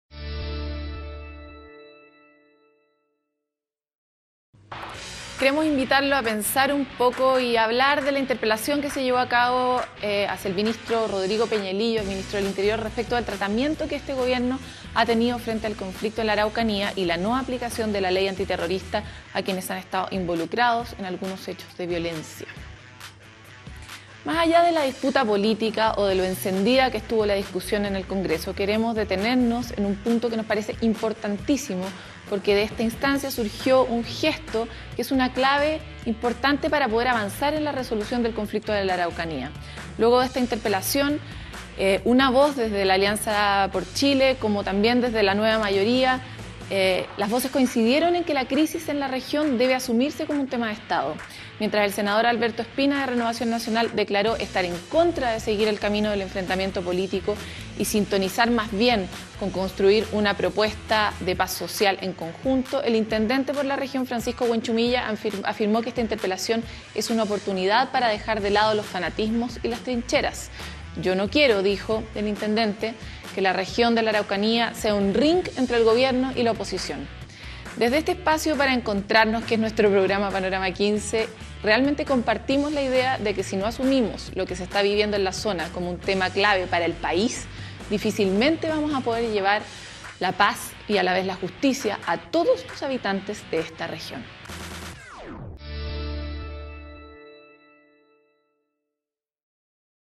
EDITORIAL P15 SOBRE INTERPELACIÓN A MINISTRO PEÑAILILLO
Queremos invitarlos a pensar un poco y a hablar de la interpelación que se llevó a cabo hacia el Ministro del Interior Rodrigo Peñailillo, respecto del tratamiento que este Gobierno ha tenido frente al conflicto en la Araucanía y la no aplicación de la ley antiterrorista a quienes han estado involucrados en algunos hechos de violencia.